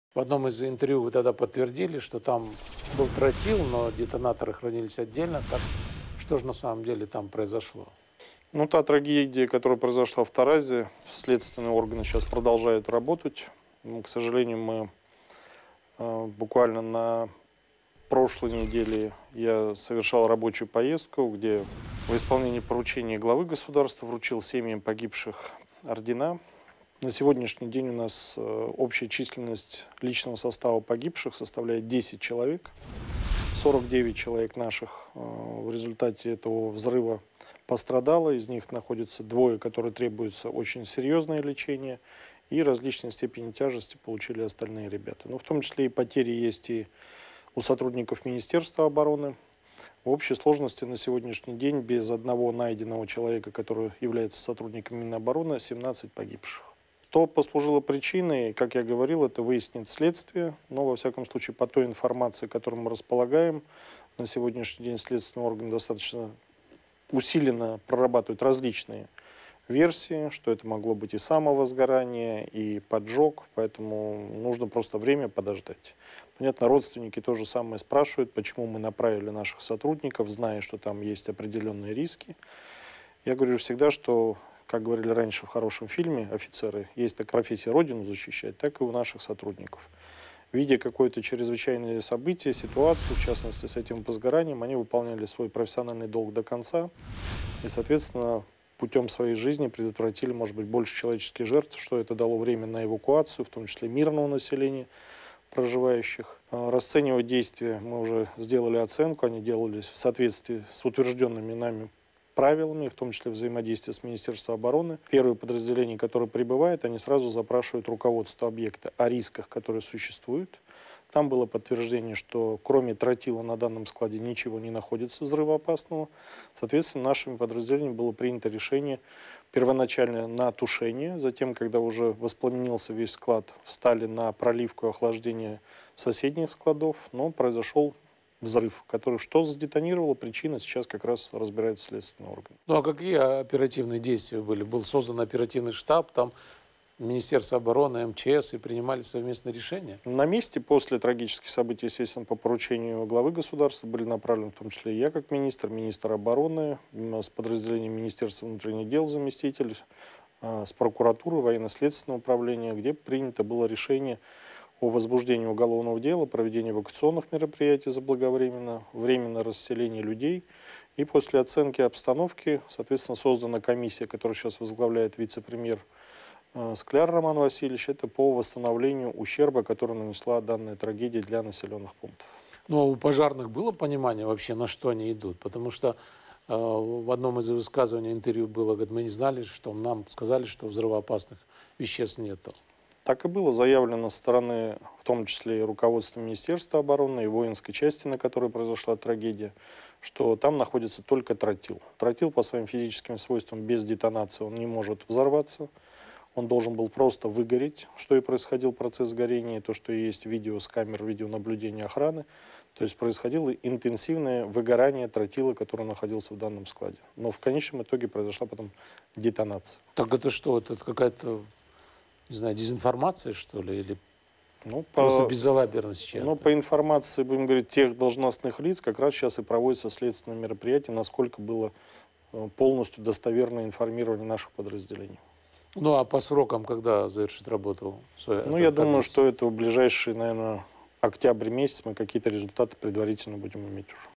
Трагедия в Таразе: знали ли пожарные на что идут
Спасатели выполняли свой долг до конца, тем самым предотвратив большее количество жертв.
В  результате ликвидации взрыва в Таразе погибли 10 служащих ЧС личного состава, также насчитывается 49 пострадавших, среди которых которых двое нуждаются  в серьезном лечении. Общее же число погибших без одного пропавшего военнослужащего-сотрудника МО РК, составляет 17 человек.